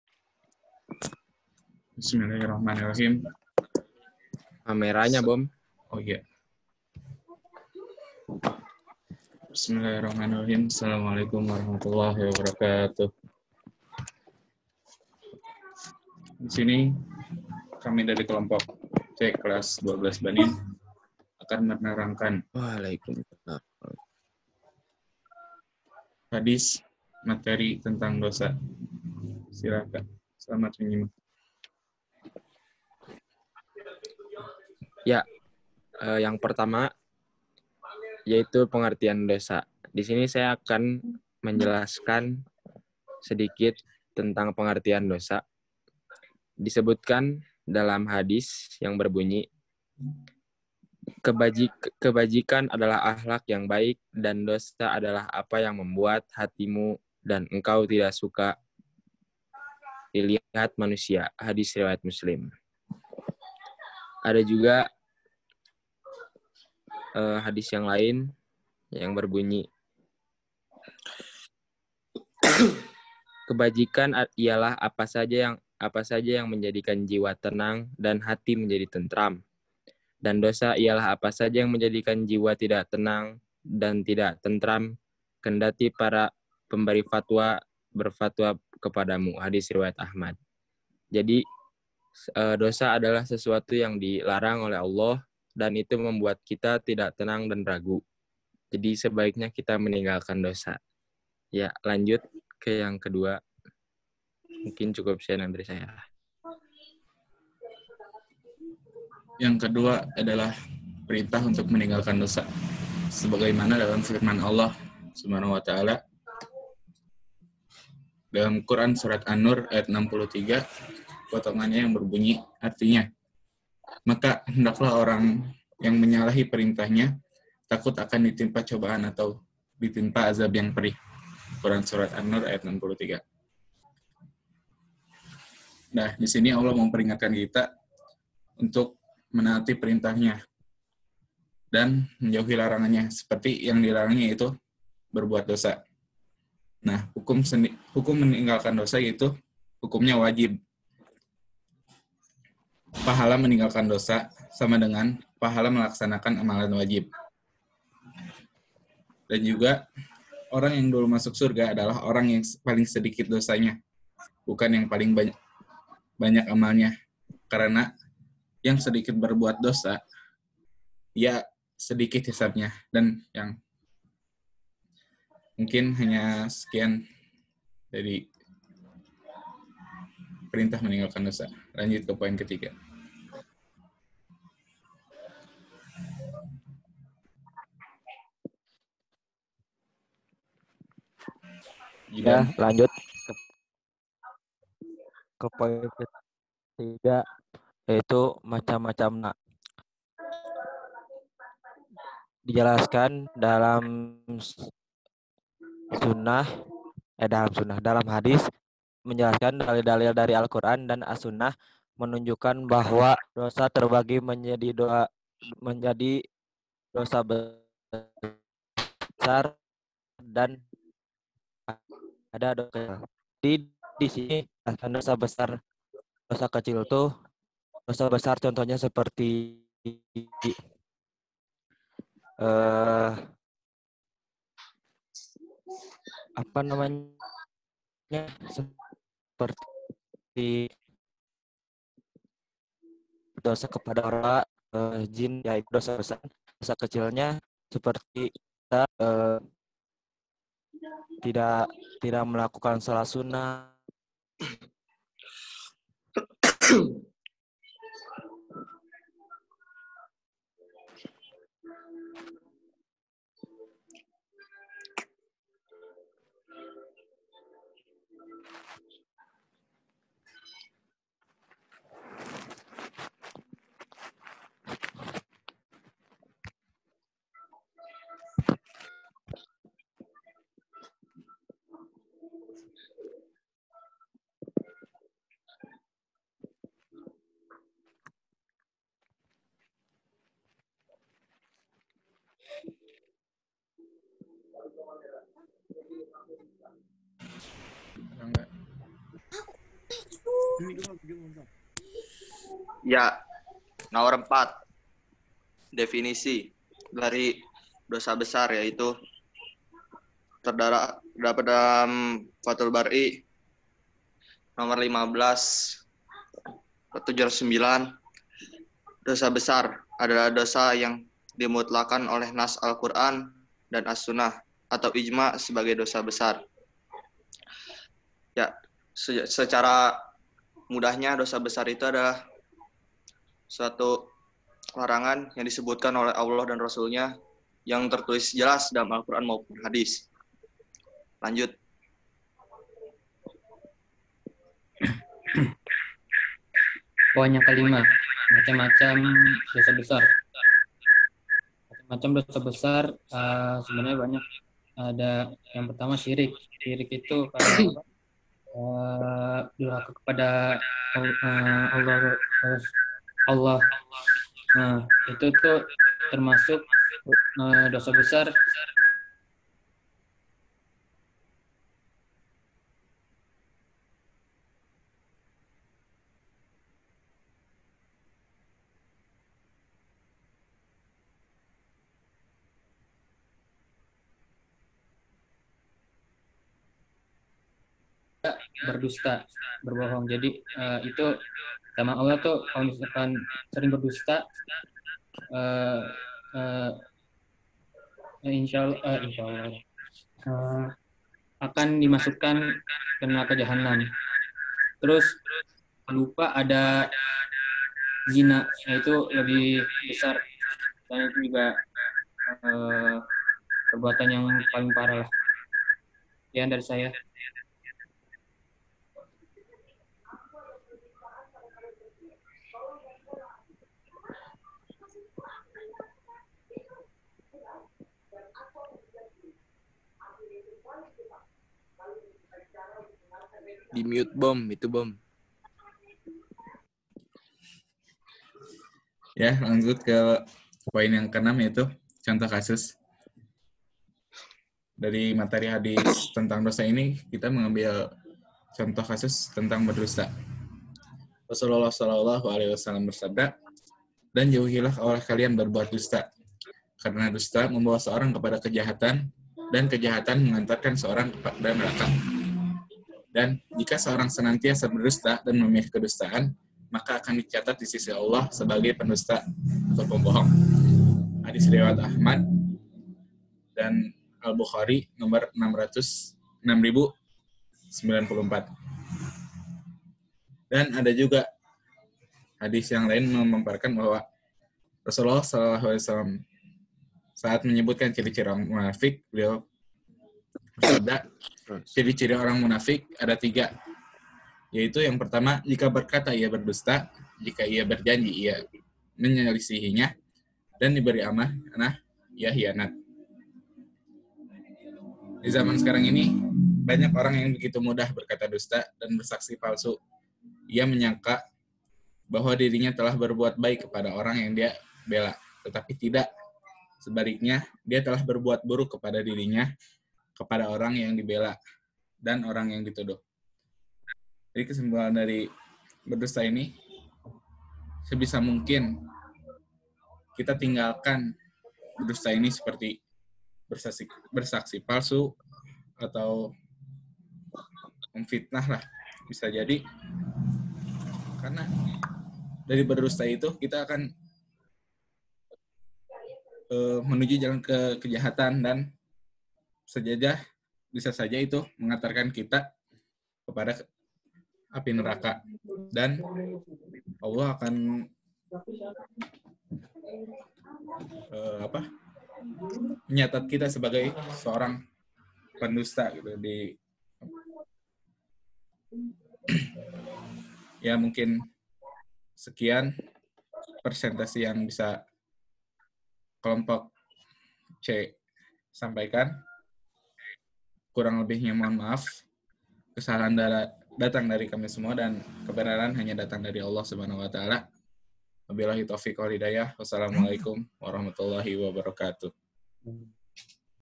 Presentasi Dirosah Kelas XII Ikhwan Kelompok C - 2020
Video ini merupakan salah satu tugas mata pelajaran Dirosah yang merupakan penjelasan materi rangkuman materi

Anggota Kelompok:
Raihan R
Ghazali
Zidane
Fathan
Rangga